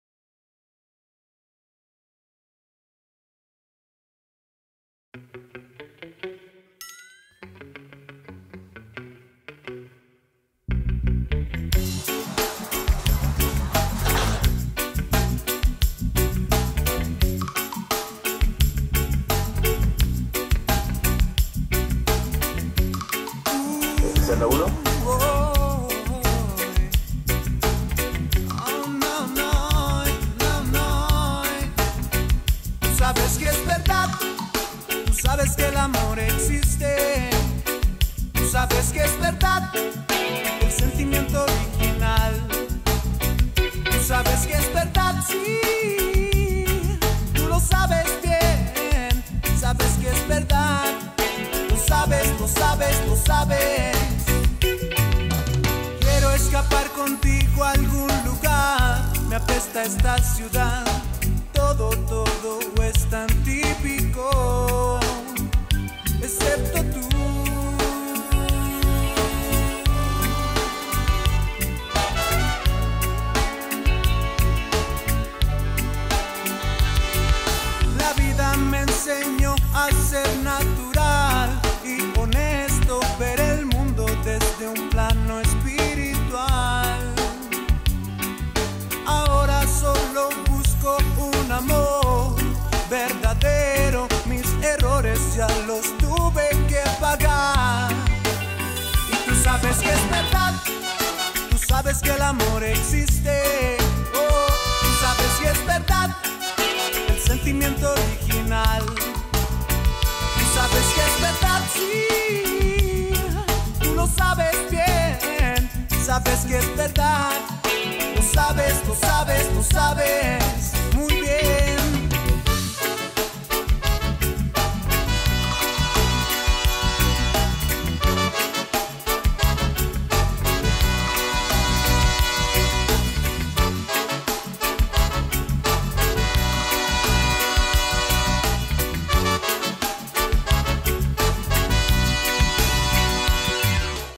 GONDWANA 30TH ANNIVERSARY TOUR
We shot video & photography for Gondwana 30th Anniversary World Tour at Martina Beach Club in Playa del Carmen at the end of 2017.

Directed, Filmed and Edited by: Josef Rios
Production Assistant: Pau Vargas
Produced by: Digital Riots